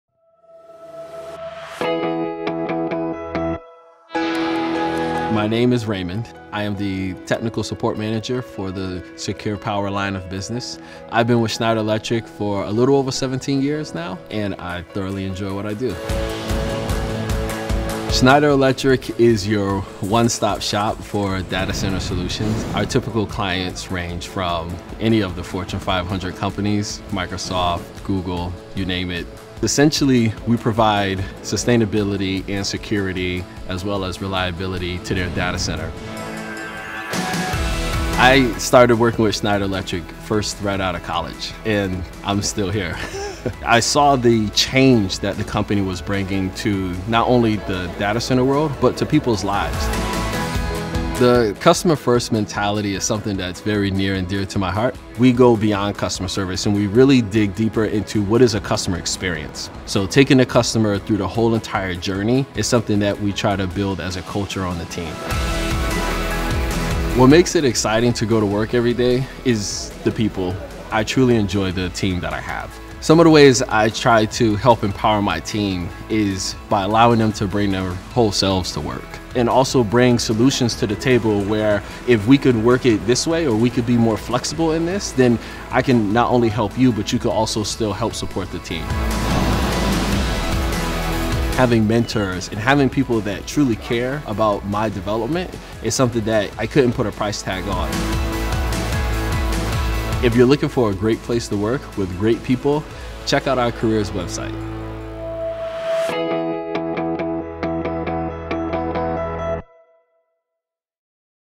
Schneider Electric Stories feat. Raymond
In this testimonial video for Schneider Electric, we spotlight Raymond, the Technical Support Manager for the company's secure power line. As Raymond narrates his experience, audiences watch a captivating blend of aerials, stock footage, and custom interviews that vividly visualize Raymond's experiences working with top-tier clients like Microsoft and Google, and emphasizing Schneider Electric’s unparalleled commitment to sustainability, security, and reliability.

Client Goals:
The client commissioned a series of testimonial videos to introduce prospective hires to the Schneider Electric company culture and employee value proposition. Raymond's interview highlighted the unmatched customer-centric culture at Schneider Electric, sharing his personal evolution since joining the company straight from college. So we meticulously crafted a video production that not only showcased the company’s transformative impact on the data center world but also spotlighted its invigorating work culture and team empowerment.

Video created by:
Filmless